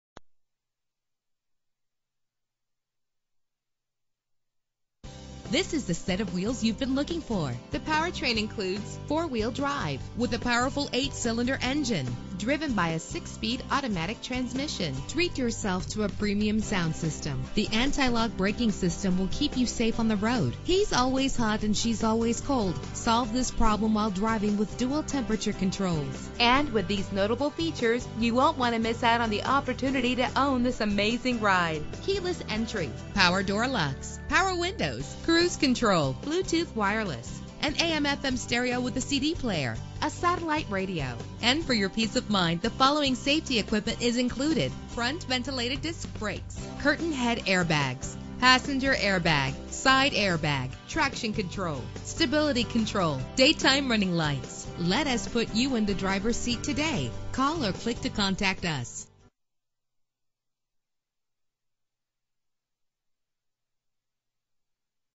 2015 Chevrolet Silverado 2500HD - Stillwater OK
Year: 2015
Make: Chevrolet
Model: Silverado 2500HD
Trim: LT
Engine: 6.0 liter 8 cylinder 16 valve
Transmission: 6-Speed Automatic
Color: Summit White
Mileage: 16
Address:
4850 W Sixth Ave
Stillwater, OK 74074